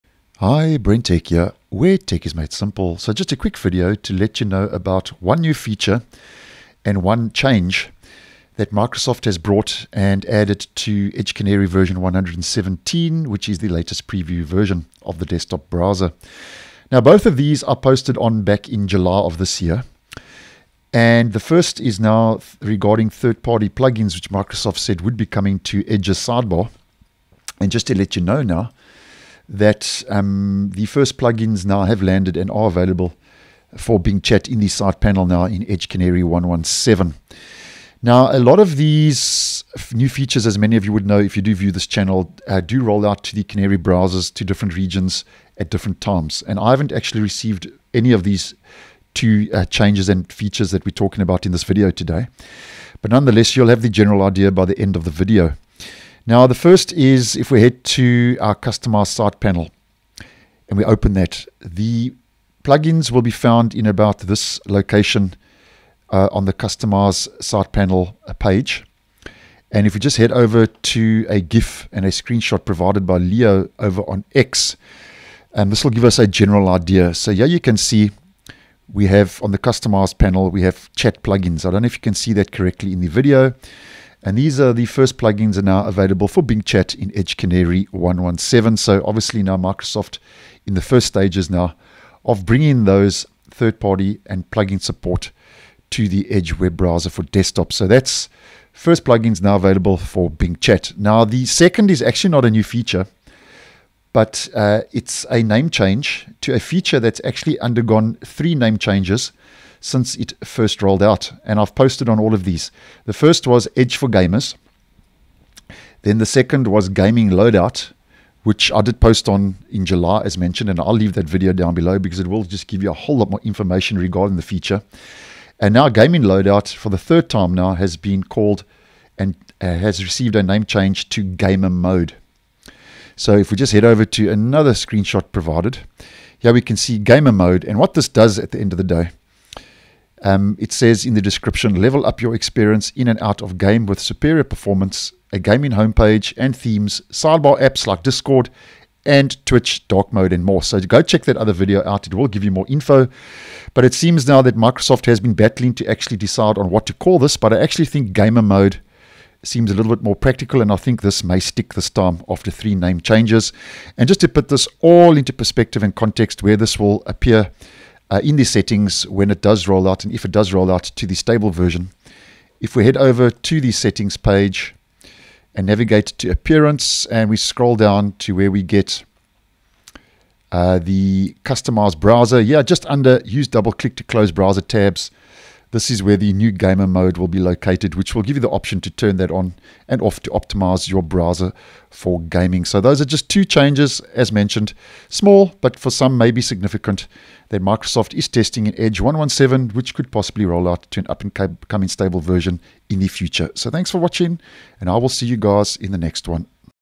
Microsoft Edge Canary 117 - New Feature and Changes
As spotted by Leopeva64 on X, the first plugins are now available for Bing Chat and previously called “Edge for gamers,” then “Gaming loadout,” this feature is now called “Gamer mode”